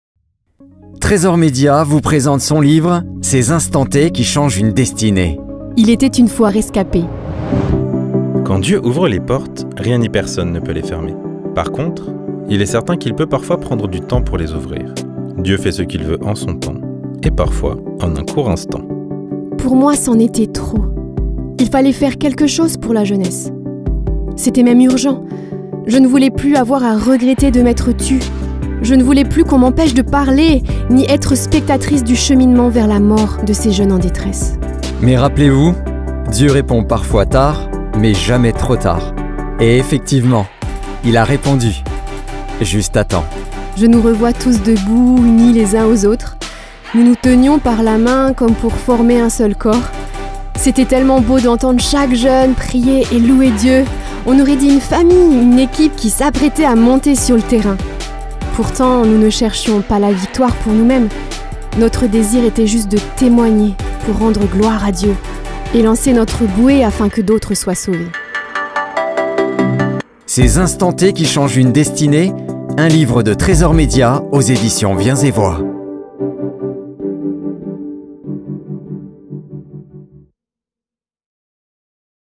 Teaser de notre livre "Ces instants T qui changent une destinée"
Découvrez le teaser de notre livre "Ces instants T qui changent une destinée" aux éditions Viens et vois.

Disponible
👉 dans nos locaux à Romans sur Isère
 👉 À la librairie Philadelphie de Mulhouse